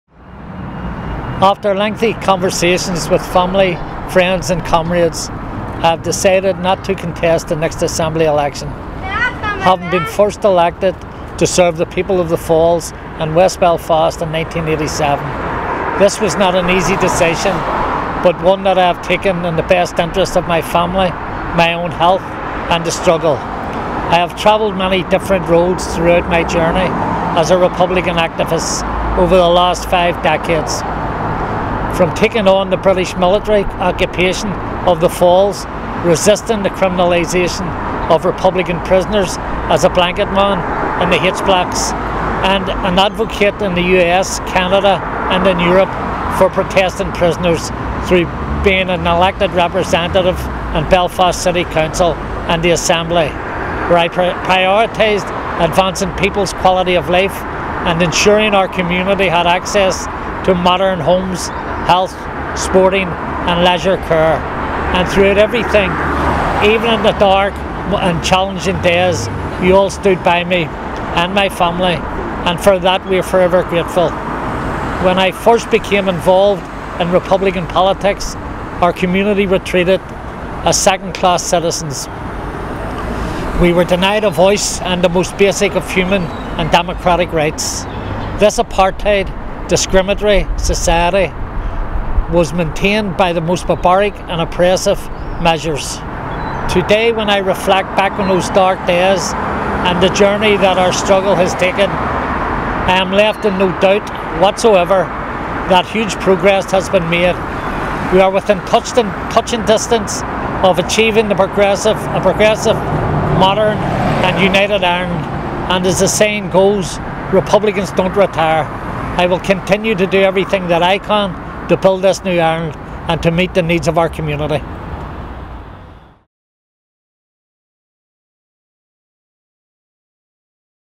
Fra McCann MLA to stand down from elected politics
Sinn Féin MLA for West Belfast Fra McCann, a veteran activist in the Irish republican struggle announces his retirement from electoral politics but pledges his continued commitment to the successful building of a new Ireland.